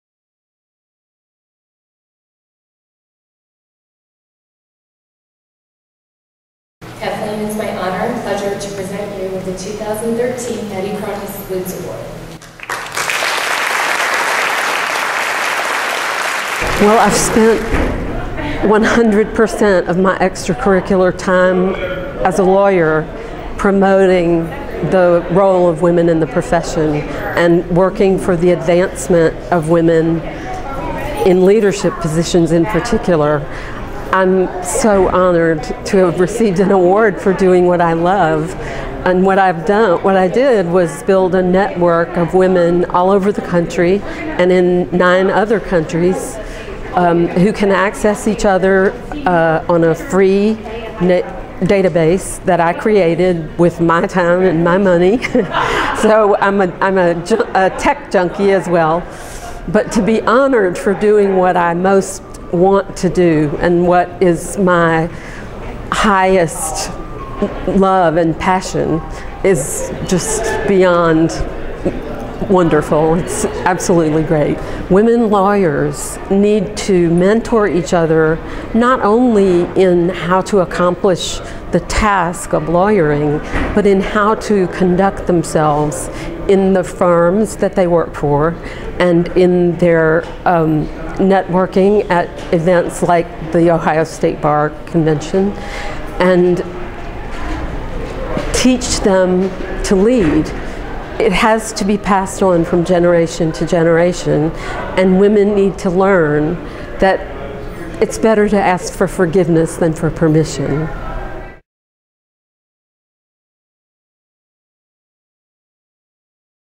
OSBA Convention 2013: Nettie Cronise Lutes Award
Kathleen Balthrop Havener of Cleveland is the recipient of the Ohio State Bar Association's 2013 Nettie Cronise Lutes Award.